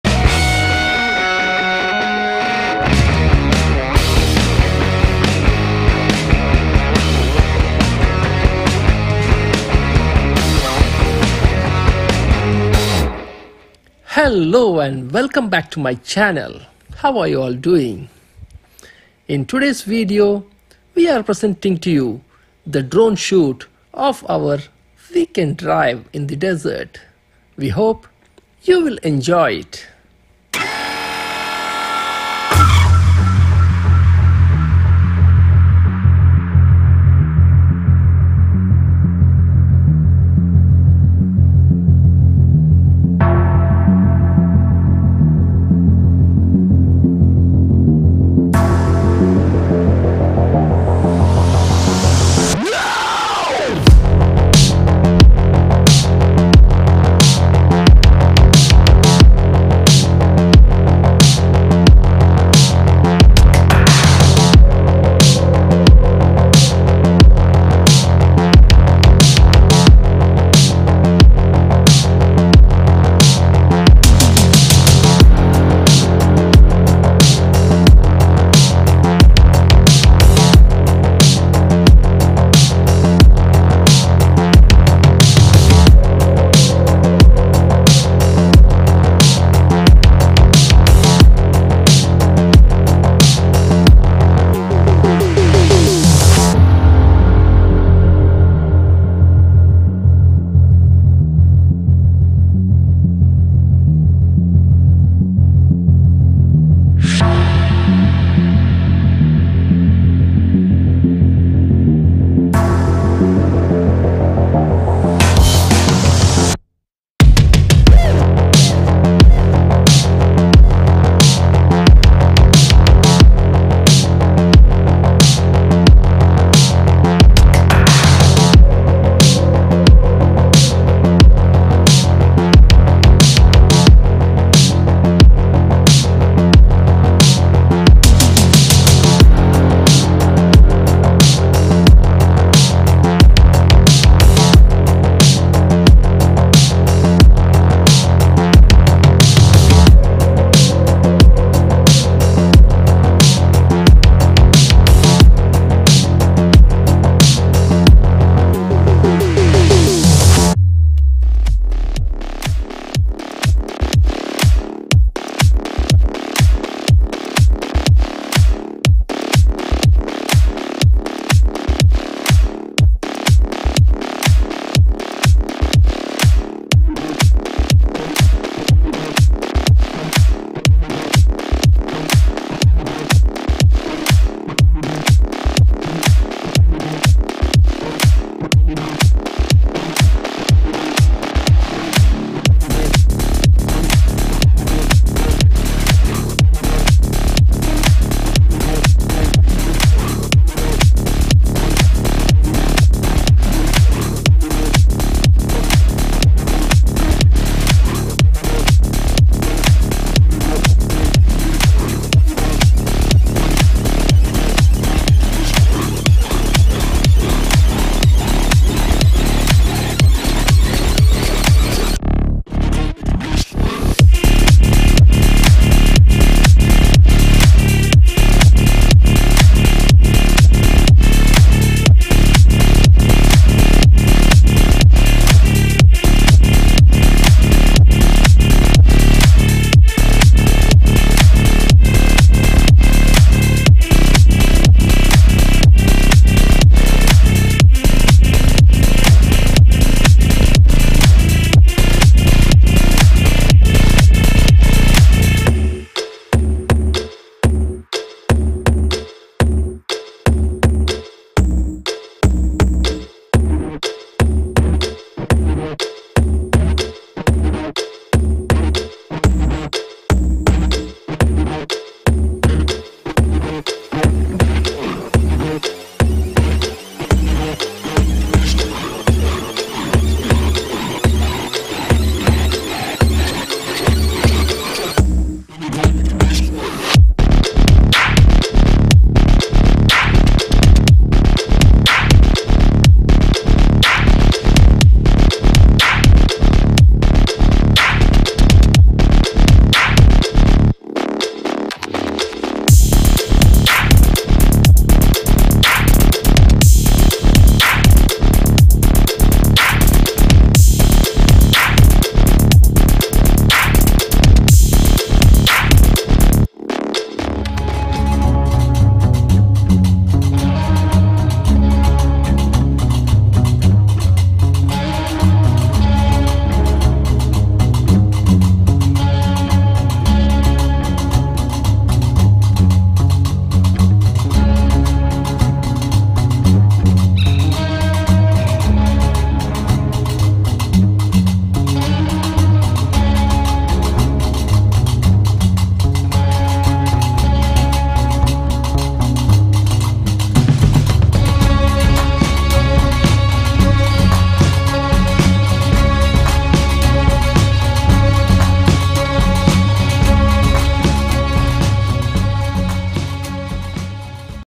Drone Shot - Big Boys & Toys | Fortuner | FJ Cruiser | Prado | Cherokee - Weekend with AOR Bros
A drone shot of weekend desert trip at Dammam Desert by Arabian Off-Roaders Team. Watch the beautiful drone video.

VC: Ahmed Mehluf